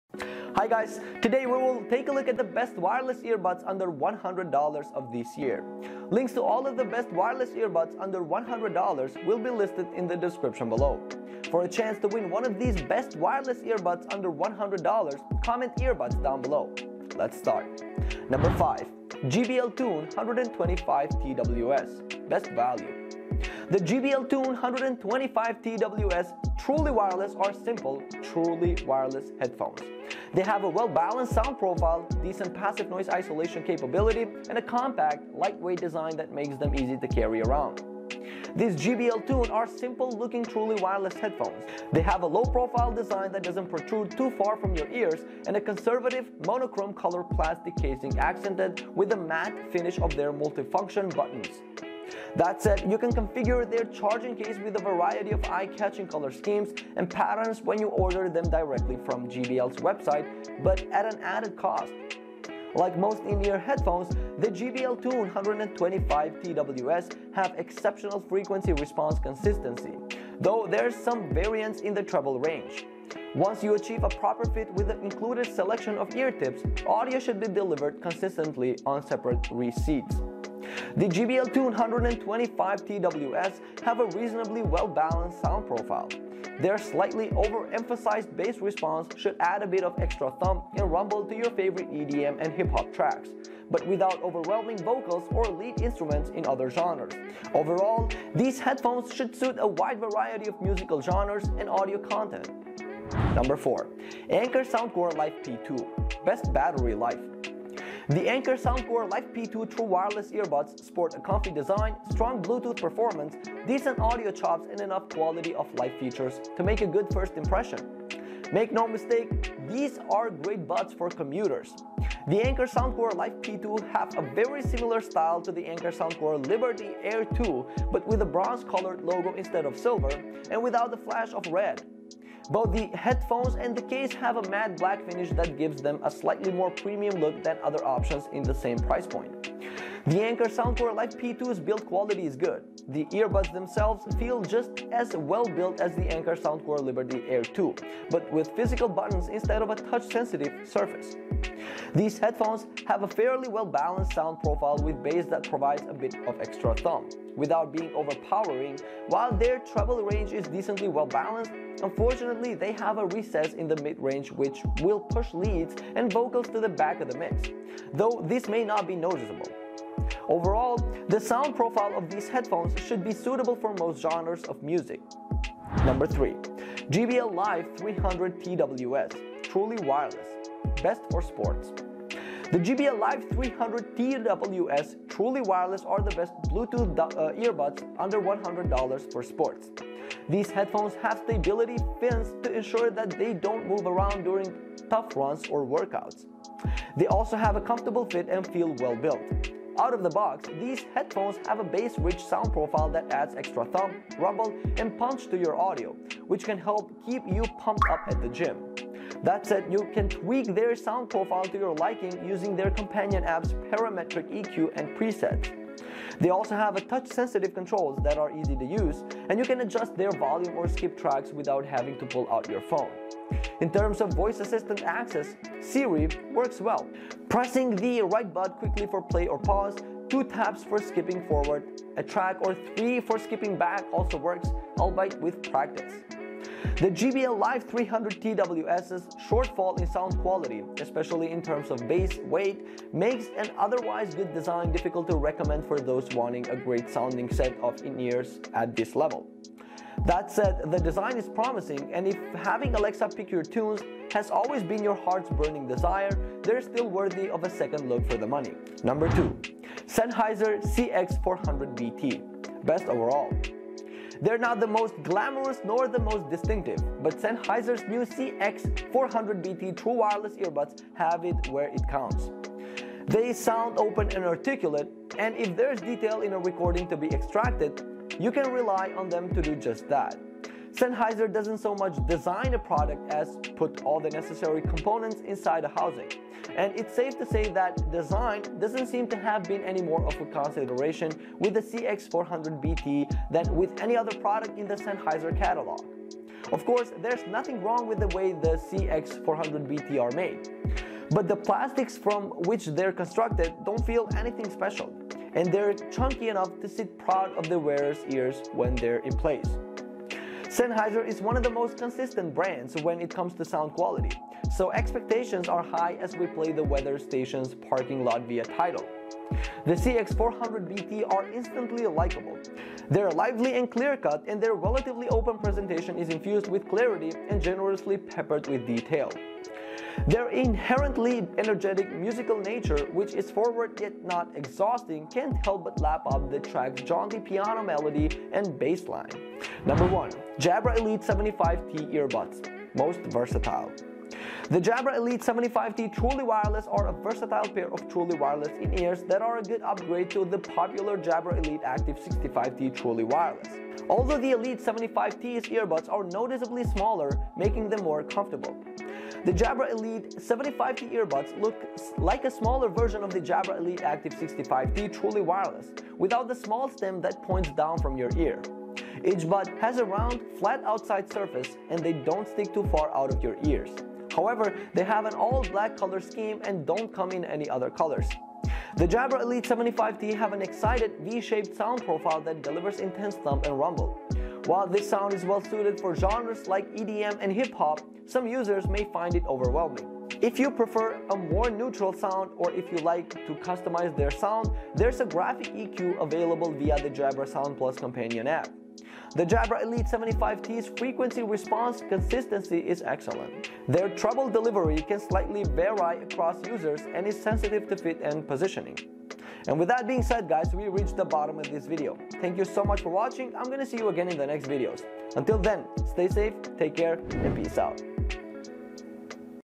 Best Wireless Earbuds Under $100 in 2022 (Top 5 Picks Reviewed)
► Links To The Best Wireless Earbuds Under $100 we listed in this video:

► 5. JBL Tune 125TWS
► 4. Anker Soundcore Life P2
► 3. JBL LIVE 300TWS Truly Wireless
► 2. Sennheiser CX 400BT
► 1. Jabra Elite 75t Earbuds